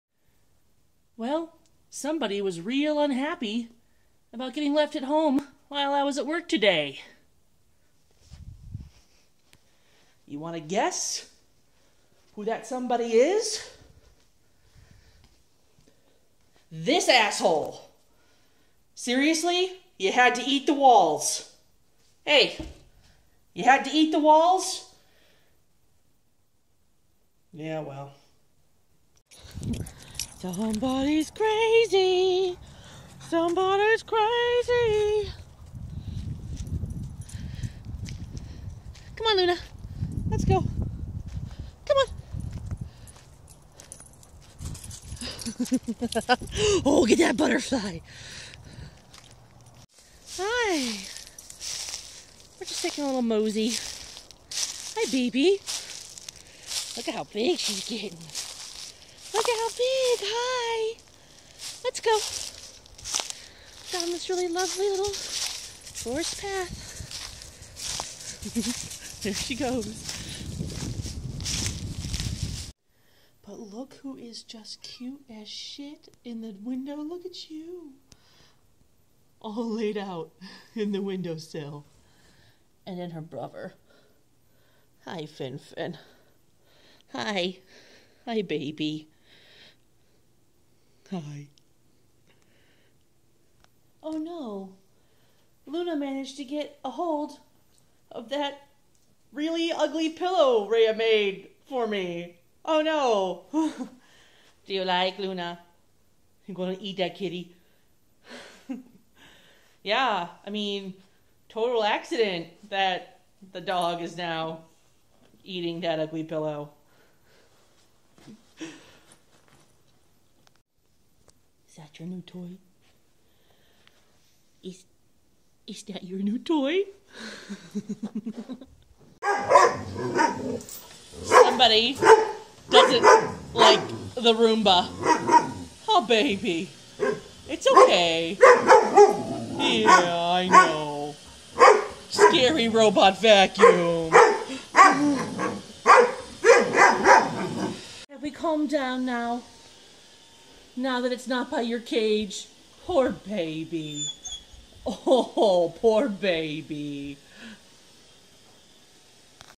The going-ons of La La Luna de Lupa Sadik, Anatolian Shepherd Puppy Extraordinaire, and Others
She's a bad dog, but then we walk it out. Plus some kitty surprises.